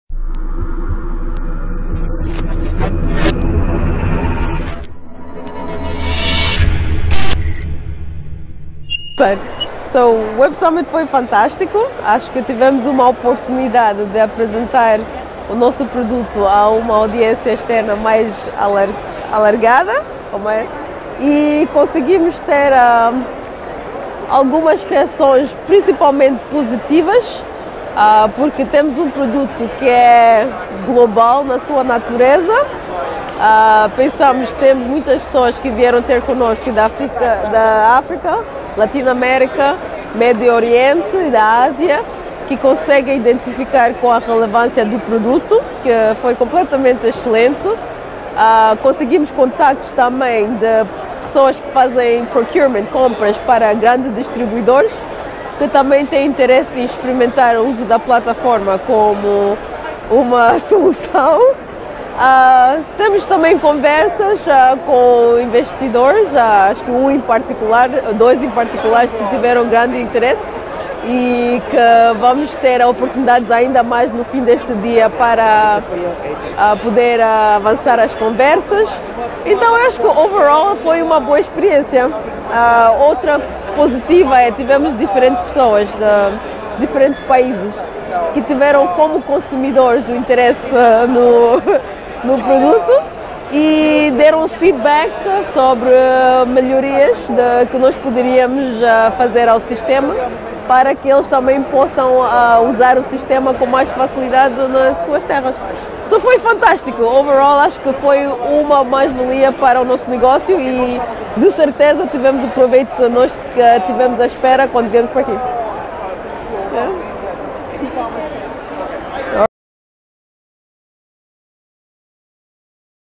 Roque Online na Web Summit 2019
Geraldine Geraldo, CEO da Roque Online partilha a sua experiência durante a Web Summit 2019.

A Startup Angolana foi a vencedora do Seedstars Luanda 2019 e representará Angola na edição global.